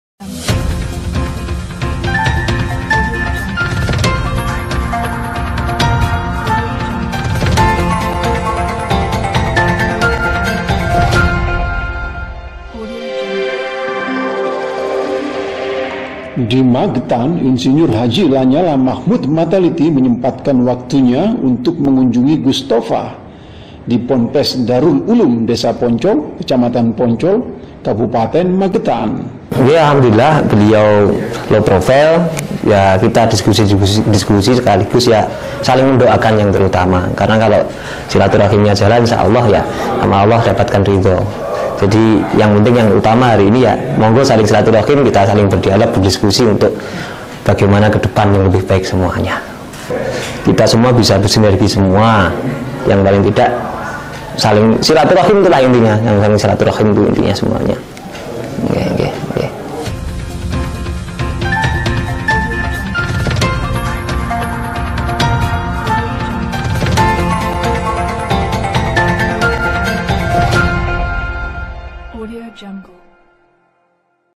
LA NYALLA BARENG GUS THOFA PONPES DARUL ULUM MAGETAN
LaNyallaJatim 1. Silaturrahim ke Gus Thofa, Ponpes Darul Ulum Poncol - Magetan